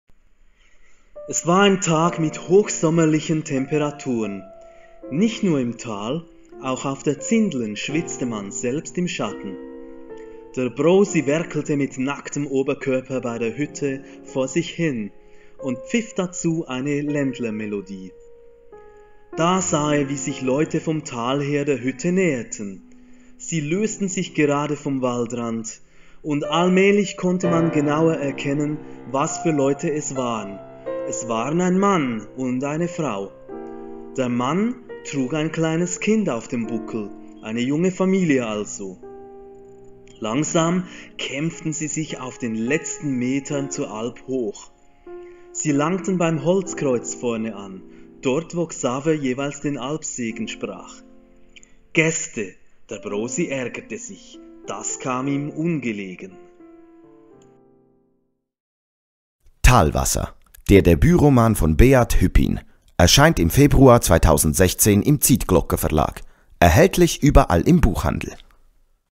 Talwasser: Zindlen (Teaser 2/7)
Talwasser: Der Debütroman von Beat Hüppin beim Zytglogge Verlag. Jede Woche bis zur Buchveröffentlichung wird ein weiterer Teaser hochgeladen.
Die Textstücke werden vom Autoren selbst gesprochen, die Buchankündigung am Ende spricht Delio Malär (vielen Dank!).